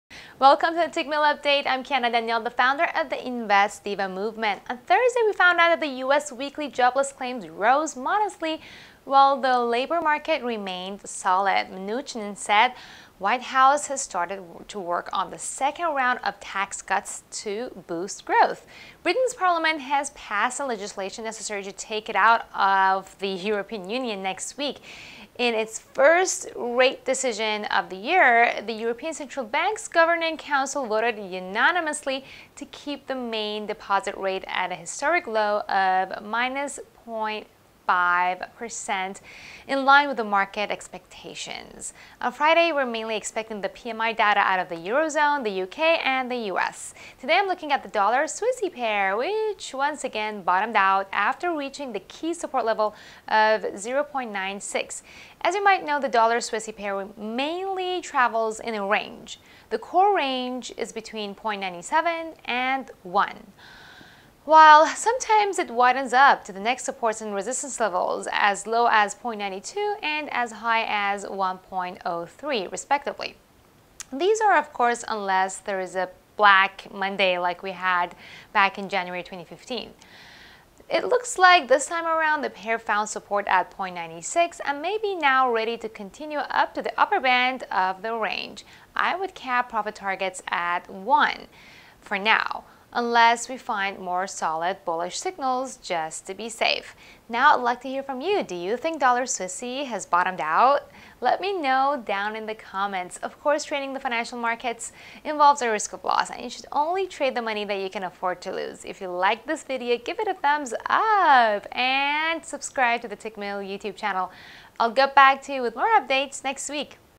January 24th, 2020 - USDCHF Breakdown
TGIF! Today we’re keeping an eye on PMI data out of the Eurozone, the UK and the US. Meanwhile, our Investing Diva will be checking out the USDCHF's latest price movement in today's video.